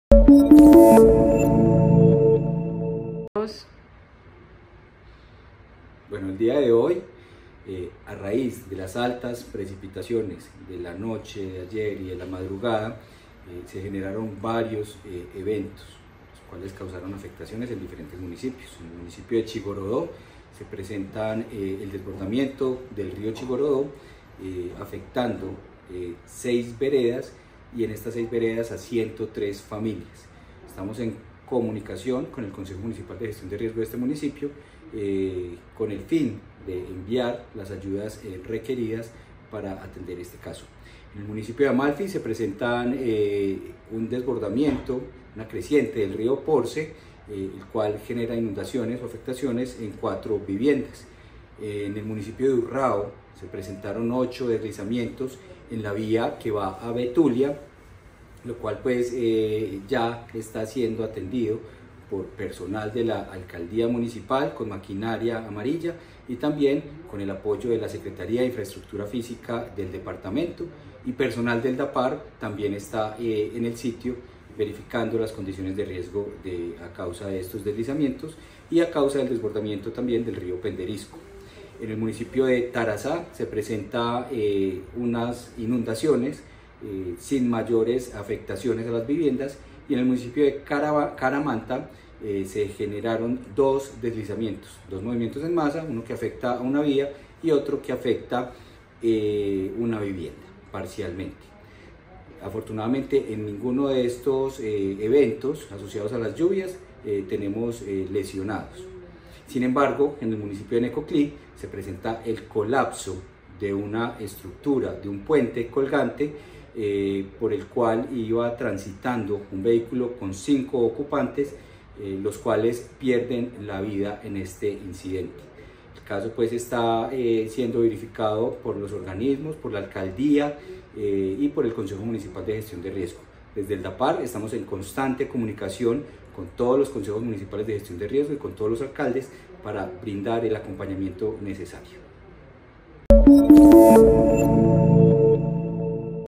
Inundaciones, movimientos en masa y avenidas torrenciales reportadas Antioquia en las últimas horas
Director Dapard, Jaime Enrique Gómez Zapata